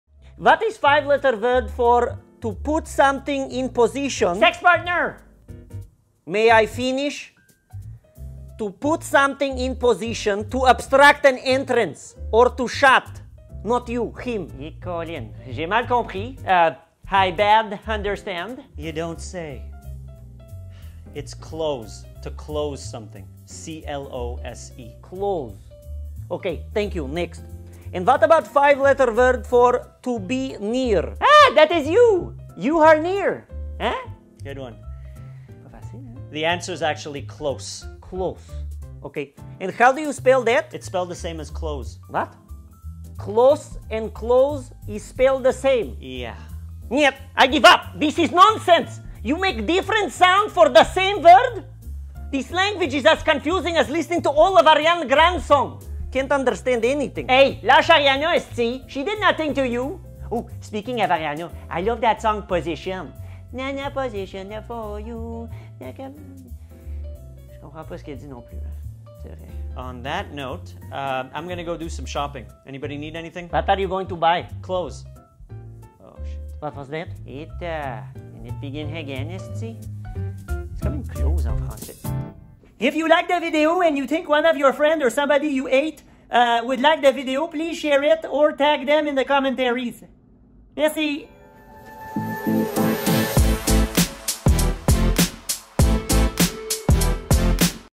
CASE CLOSE! - English is confusing AF
Oleg continues to attempt to learn English by doing crossword puzzles.
Unfortunately, he find himself in a mess of confusion and frustration when English words are used in the most random ways!